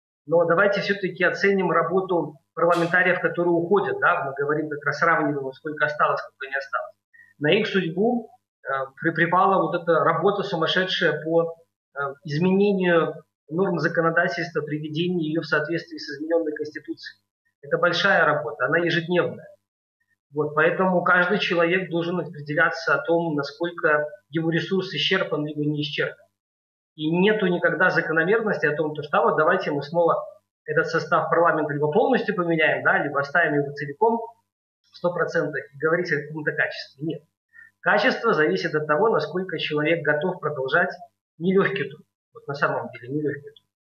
Итоги работы седьмого созыва Палаты представителей Национального собрания
Виктор Воробьев: «Давайте оценим работу парламентариев, которые уходят. На их судьбы выпала работа по изменению норм законодательства и приведение его в соответствие с обновленной Конституцией. Это большая работа, она ежедневная».

Итоги работы седьмого созыва Палаты представителей Национального собрания в нашем эфире подвел кандидат юридических наук Виктор Воробьев.